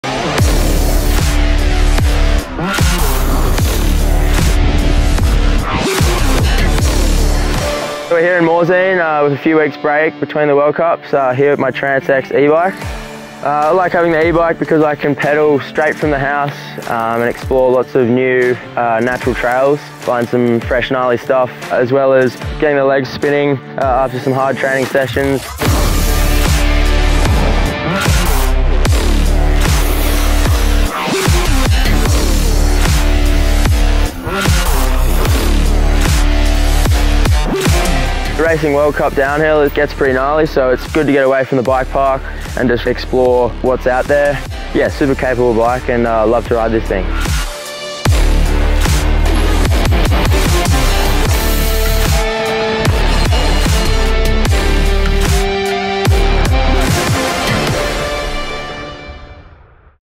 🔥 Luke Meier-Smith fait parler ses talents de descendeur en Trance X Advanced E+ Elite à Morzine !
Luke Meier-Smith, athlète Giant champion de DH et d'enduro, prend du bon temps à Morzine au guidon de son VTT Electrique léger Trance X Advanced E+ Elite !